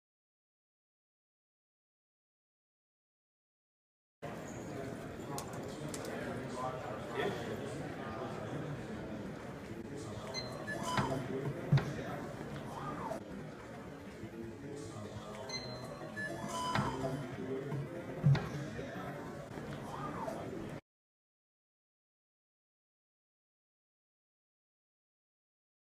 Orion Turnstile Entry
This short video demonstrates the Orion Entrance Control ODAB turnstile with the IBC card reader scanner.